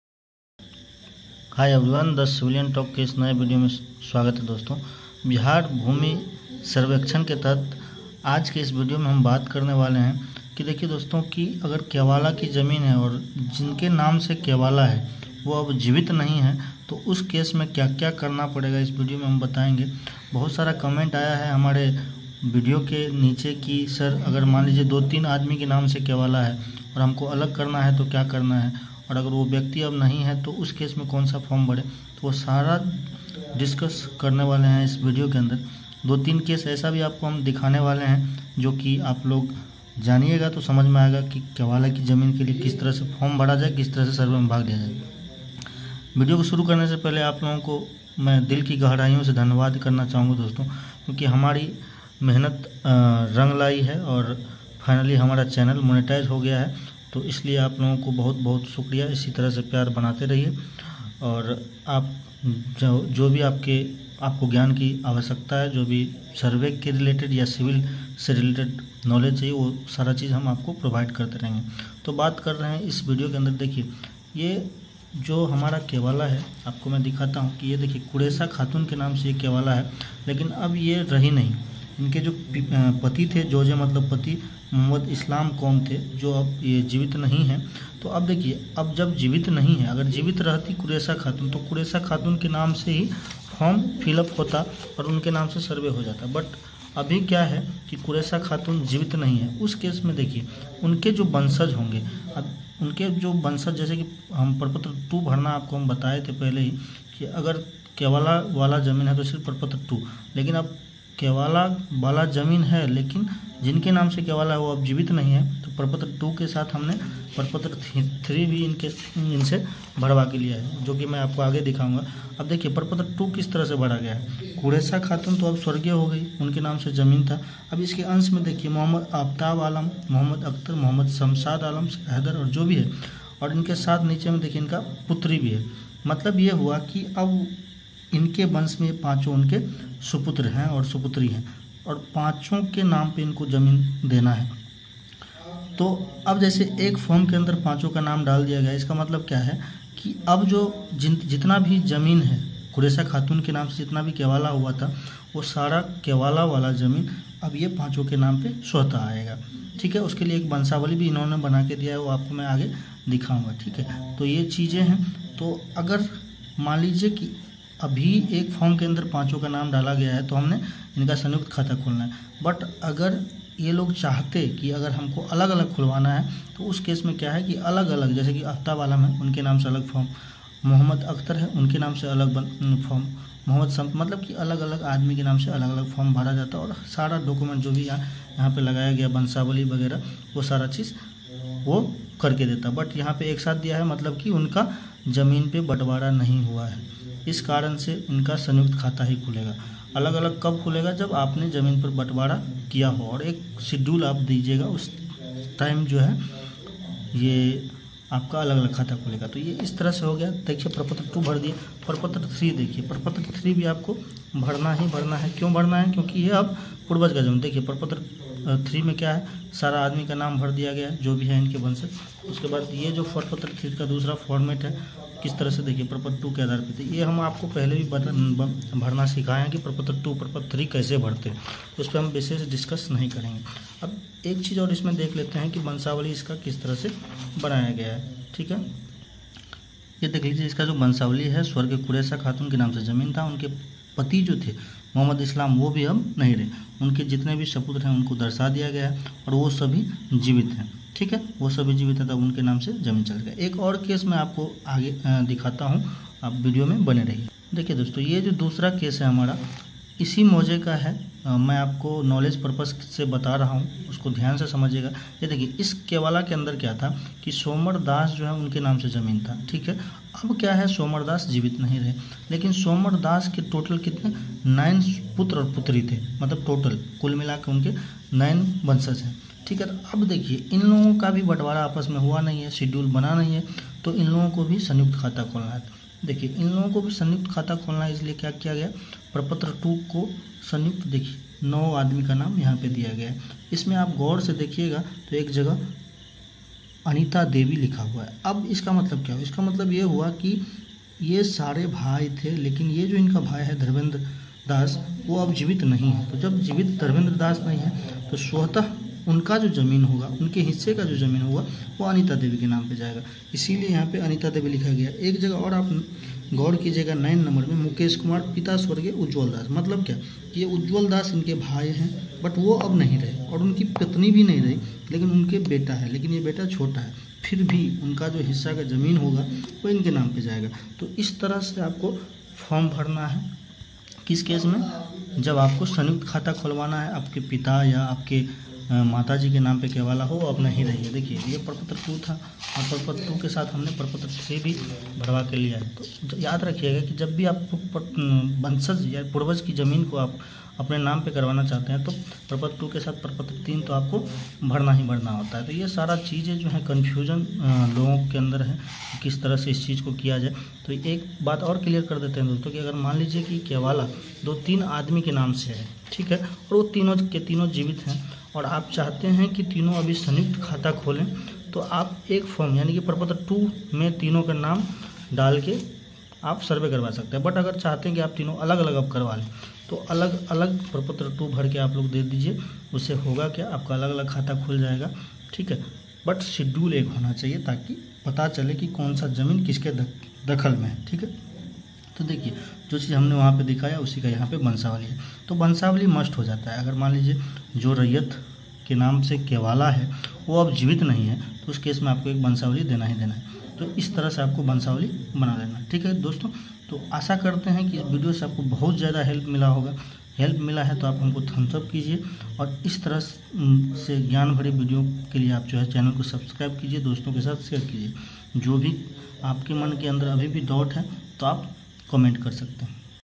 केवाला की जमीन, पूर्वज की केवाला की जमीन का सर्वे कैसे होगा ,कौन सा दस्तावेज लगेगा 🤔जानिए।
इस वीडियो के माध्यम से हमने बताया है कि अगर केवल की रैयत जीवित नहीं है तो उसे कैसे में जो उसके जमीन का सर्वे होगा जस्ट लाइक खटियाणी जमीन के जैसा होगा मतलब की खटियाणी जमीन में जिस तरह से मंशावली और शेड्यूल की मांग की जाती है इस तरह से पूर्वज के केवल की जमीन के लिए भी प्रोसेस होगा इसके अलावा केवल की जमीन का जो सर्व है सिंपल प्रोसेस है जैसा कि हमने आपको पहले बताया था कि प्रपत्र दो सिर्फ और सिर्फ भरना है जिनके नाम से केवल है ठीक है अगर केवल दो-तीन आदमी के नाम से है तो अलग-अलग आप डिमांड करवाना चाहते हैं तो सब अलग-अलग फॉर्म भर के जमा करेंगे सबका अलग-अलग डिमांड खतियान बन जाएगा।